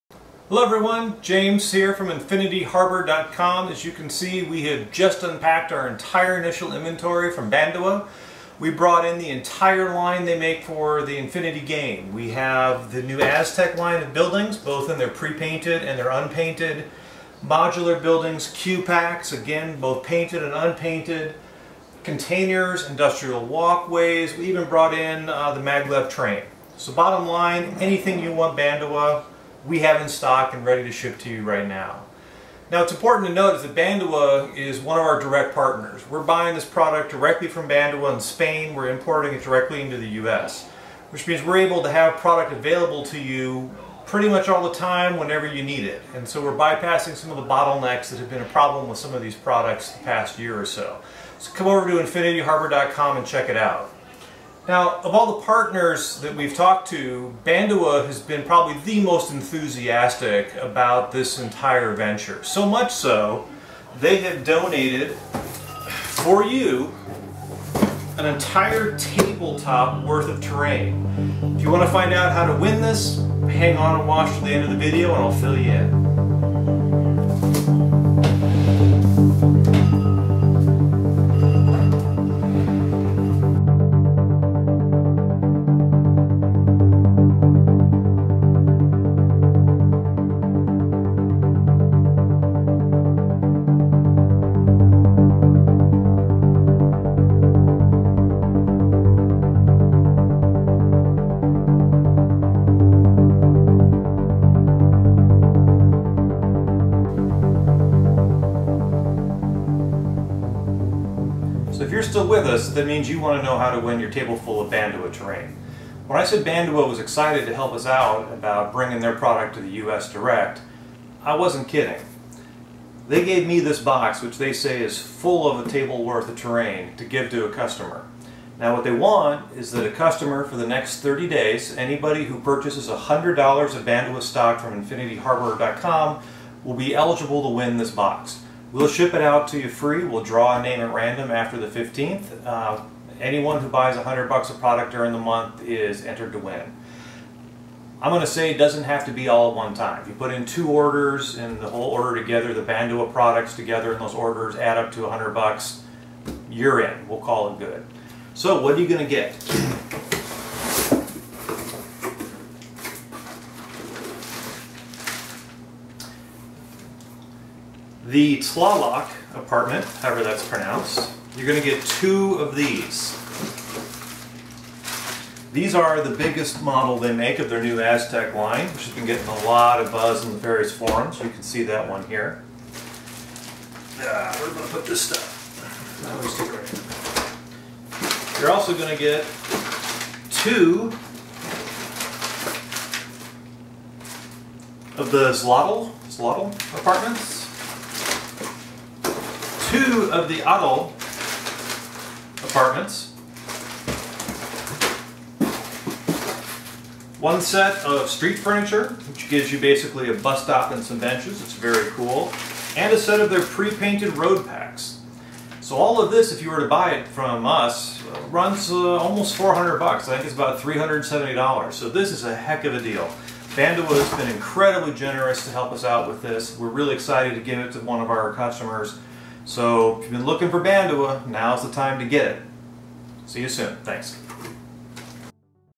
Infinity Harbour - Bandua Giveaway
Bandua has given us a box of terrain to give to one of our customers. From now until October 15th, 2015, purchase $100 of Bandua product and you will be entered to win! See the video for details. to get your Bandua!